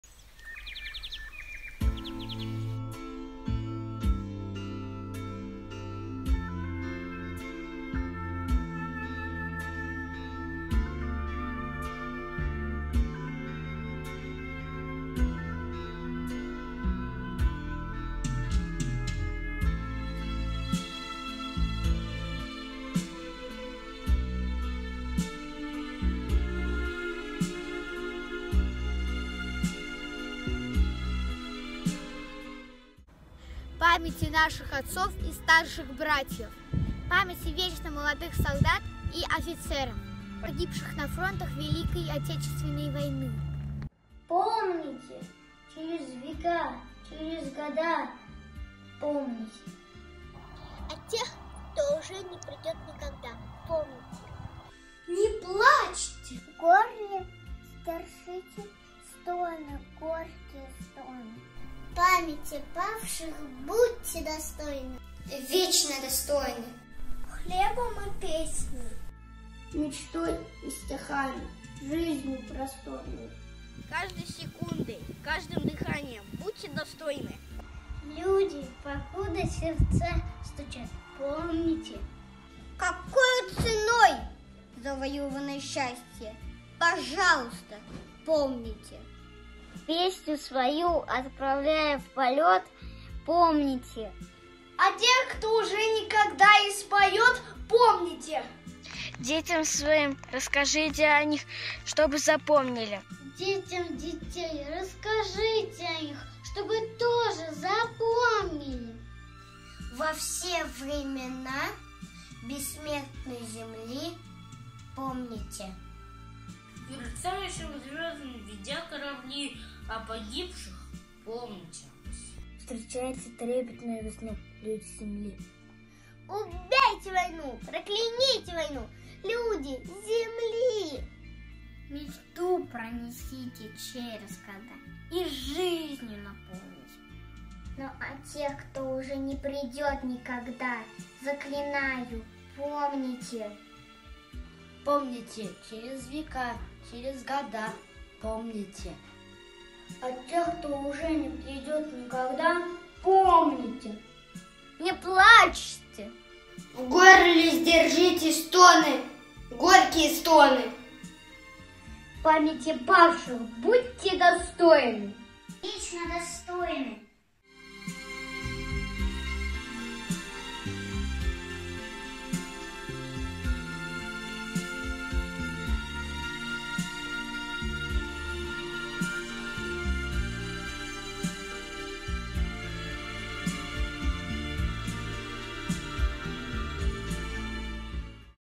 С Днем Победы💥Помните. Роберт Рождественский💥Читают дети детского сада 1945. Москва.
Очень люблю этот ролик. Он искренний. Дети из группы моей дочери читают стих «Помните» Роберта Рождественского. Ролик снят в карантин во время ковида, поэтому каждый снял там, где он находился.